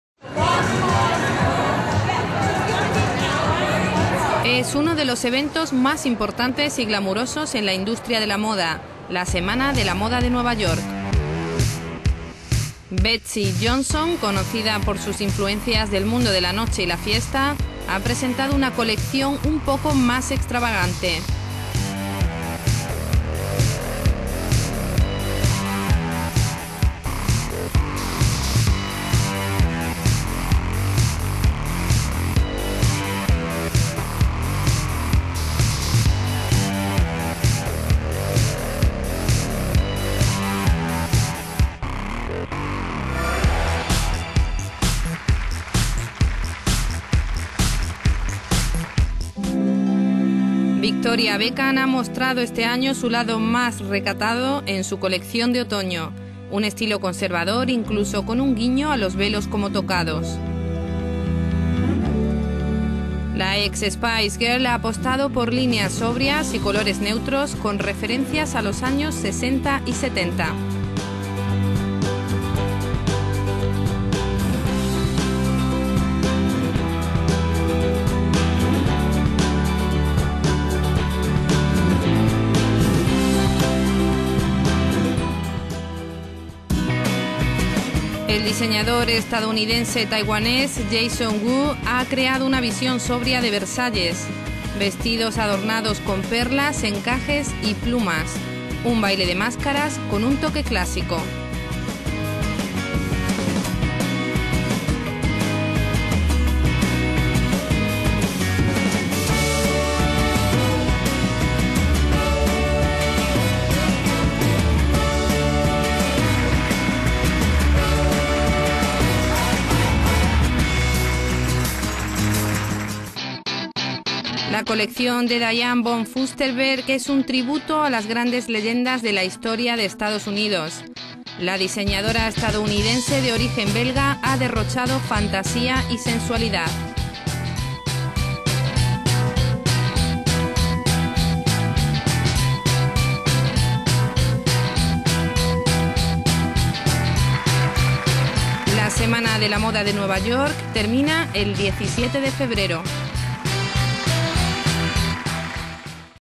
euronews le mag - Betsey Johnson y Jason Wu en la Semana de la Moda de...
Betsey Johnson y Jason Wu son algunos de los diseñadores estadounidenses que han presentado sus colecciones en la Semana de la Moda de Nueva York.